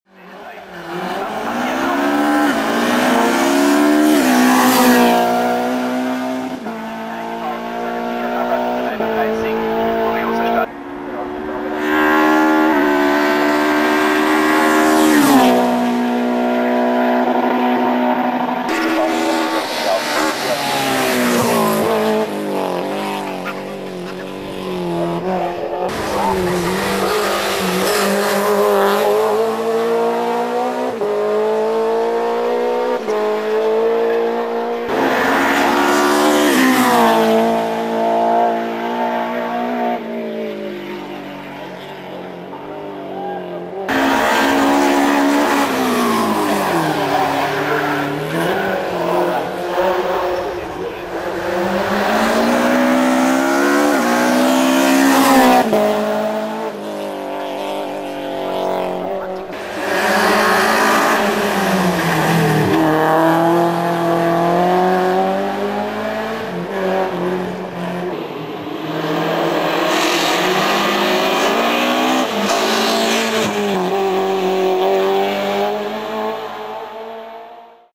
28.-29.06.2008 - XIV. ADAC Ibergrennen - #358 - Werner Jetzt - Fiat Abarth 131
Werner Jetzt mit seinem Fiat Abarth 131 beim XIV. ADAC Iberg Bergrennen in Heilbad Heiligensatdt. Videomaterial: Movie Georg - Videoschnitt: AK-Motorsportproduction. --- Fiat 131 Rallye
Der Fiat 131 Abarth wurde am 1. April 1976 für Rennen der Gruppe 4 zugelassen und konnte damit ab diesem Datum an internationalen Rennen teilnehmen. 1977 gewann der Fiat 131 Abarth den Weltmeistertitel, der 1978 wiederholt wurde.
1980: Die Rallye Monte Carlo der verschneiten, oft verregneten Rallye gewinnt Walter Röhrl, der Jahre später zum besten Rallye-Fahrer aller Zeiten gekürt werden wird, das erste Mal in Monte Carlo und erringt damit einen der wichtigsten Siege in der Renngeschichte des Fiat 131 Abarth.